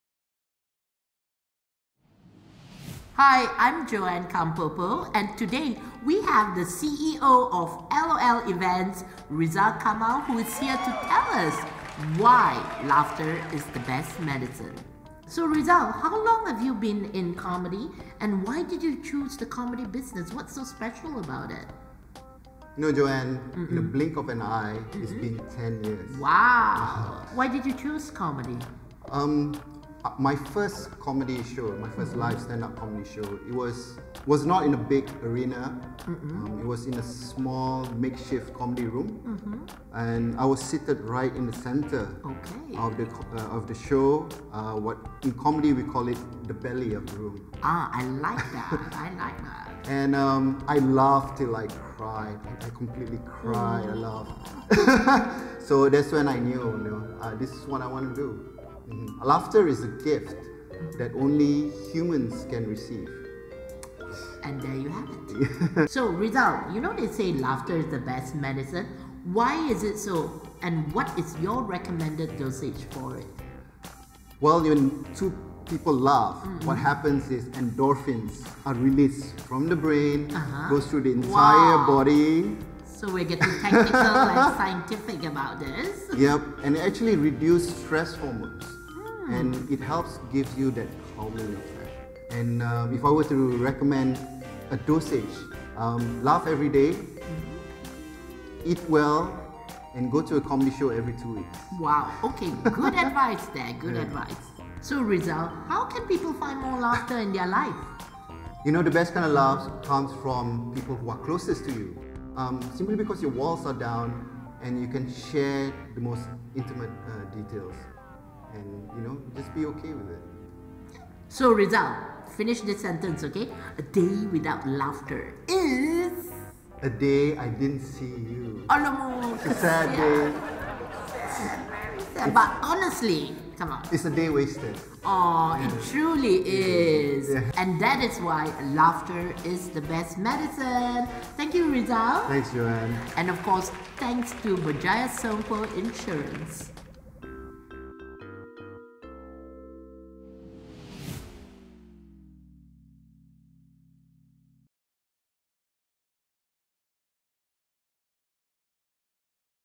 Laughter is the Best Medicine
Why is laughter the best medicine? Here’s a hint, watch the video & enter the contest to stand a chance to win a pair of tickets to the “Kevin Hart Irresponsible Tour” LIVE in Kuala Lumpur on 15 December 2018! A total of 25 pairs of tickets to be won.
Hurry, contest ends 27 November 2018.